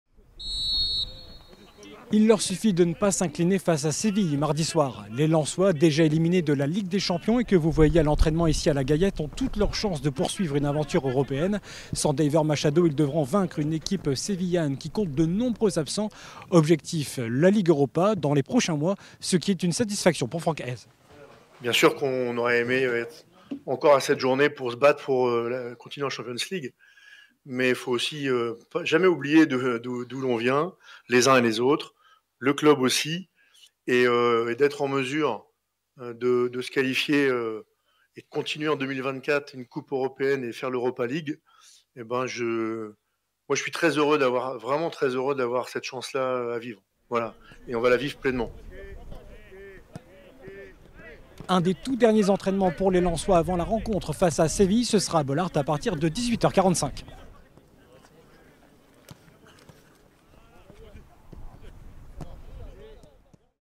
Dernier soir de Ligue des Champions à Bollaert
[ SPORT ] 🎥⚽ 𝗗𝗲𝗿𝗻𝗶𝗲𝗿 𝘀𝗼𝗶𝗿 𝗱𝗲 𝗟𝗶𝗴𝘂𝗲 𝗱𝗲𝘀 𝗖𝗵𝗮𝗺𝗽𝗶𝗼𝗻𝘀 𝗮̀ 𝗕𝗼𝗹𝗹𝗮𝗲𝗿𝘁

Face à Séville, les Lensois s'apprêtent à disputer leur dernière rencontre de Ligue des Champions et espèrent finir troisièmes de leur groupe afin de décrocher un ticket pour la Ligue Europa. Dernier entraînement et mot du coach avant le match à suivre mardi, à 18h45.